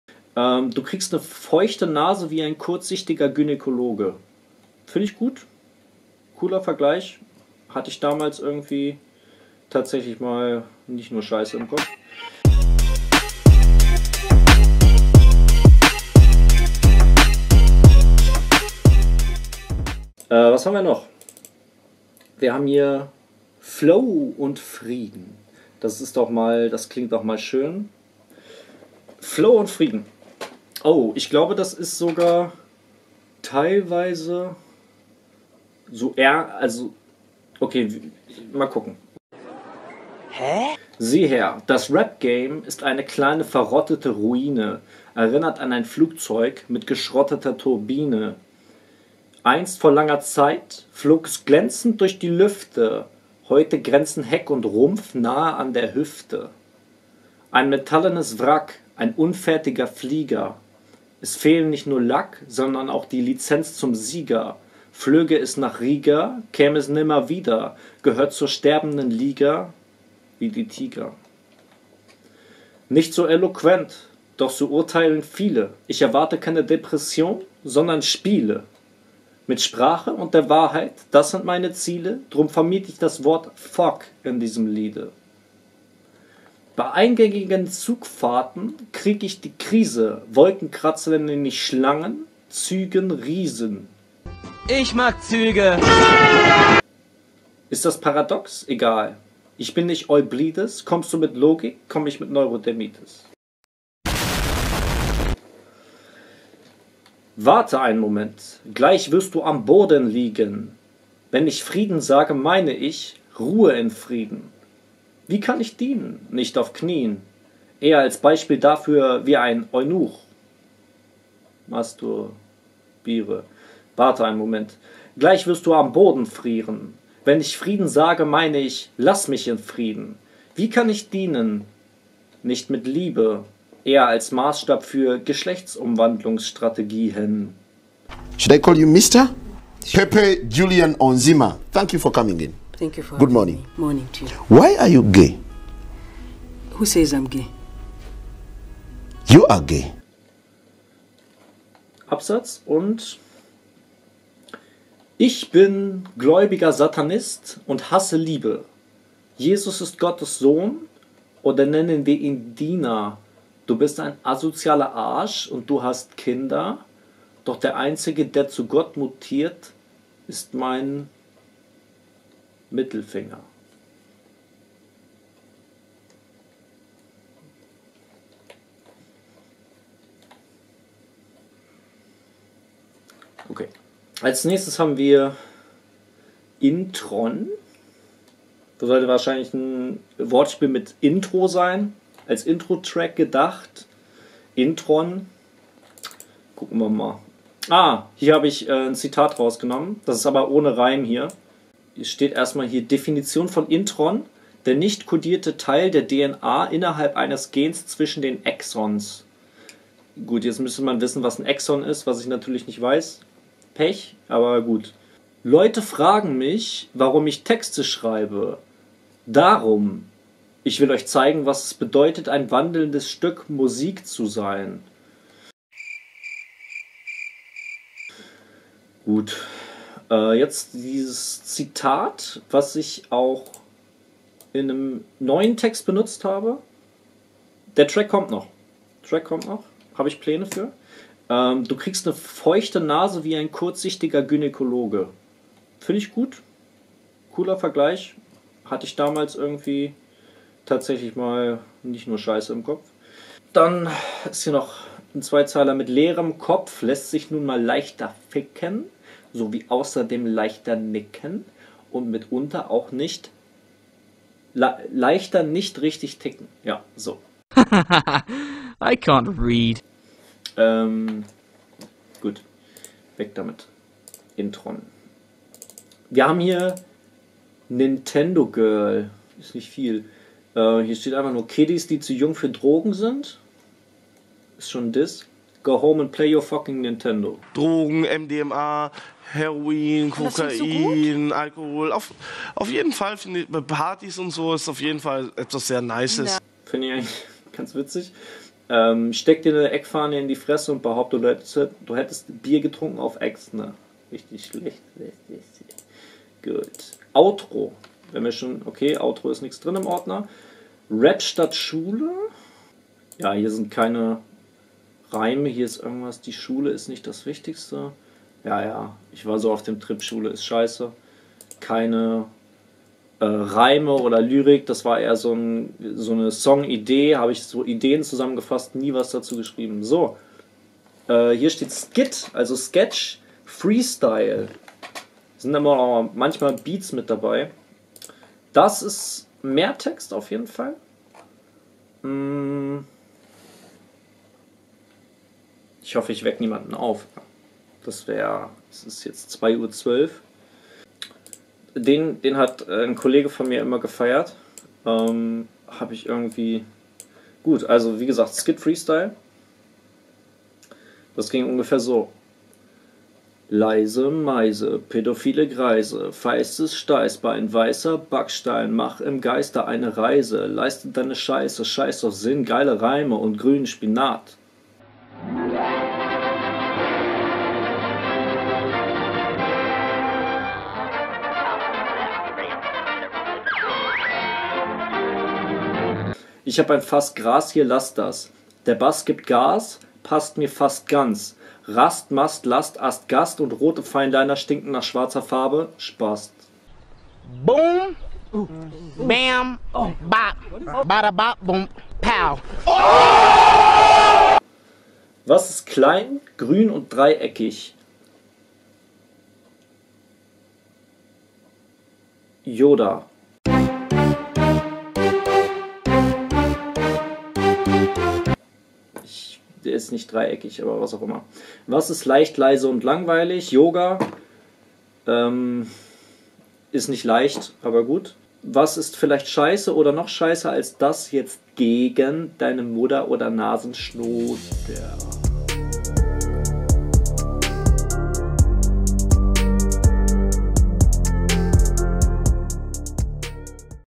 Was ist klein, grün und dreieckig? ++++ Noch MEHR alte Texte
Und eine weitere Fuhre an alten Texten. Nicht, dass sie schon Falten hätten. Doch der Cringe-Faktor ist hoch. Was dachte ich mir damals bloß?

Wenn ihr mich unterstützen wollt, abboniert gerne den Kanal und helft mir dabei, auf Youtube zu wachsen!